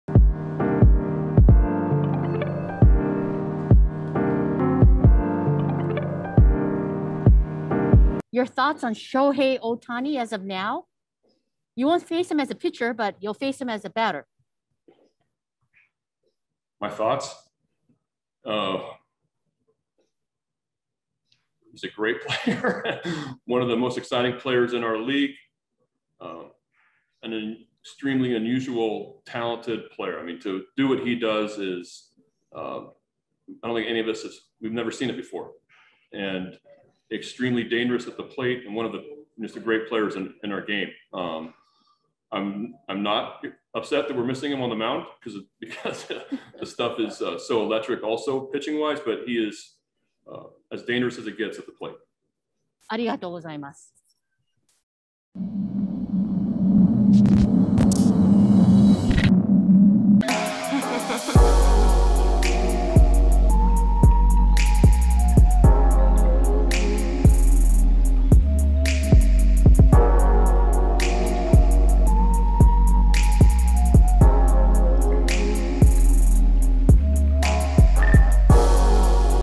Brandon Hyde Shares His Thoughts on Shohei Ohtani: "We Haven't Seen This Before" | ORIOLES
Watch as